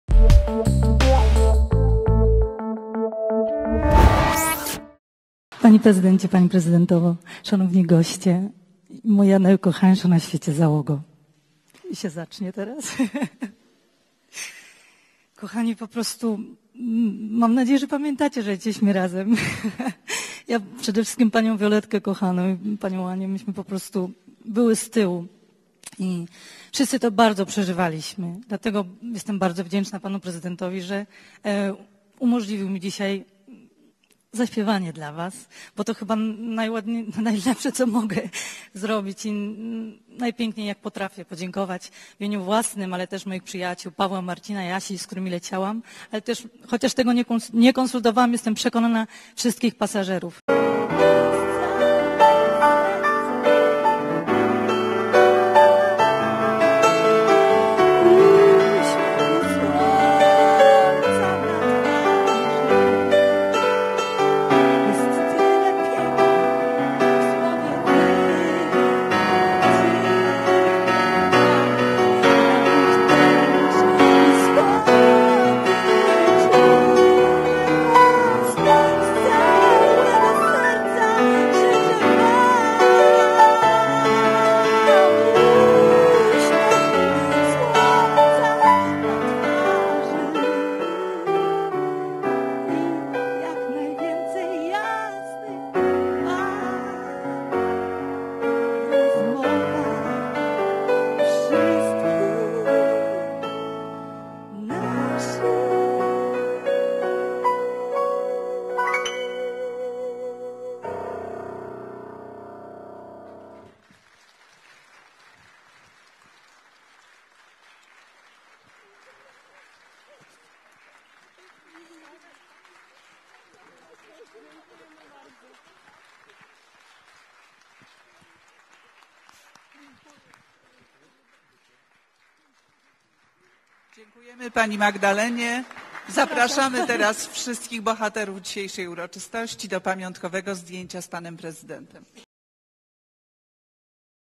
Magda Steczkowska śpiewa dla kapitana Wrony i całej załogi
gazeta.tv prezentuje: Magda Steczkowska śpiewa w Pałacu Prezydenckim. Więcej wideo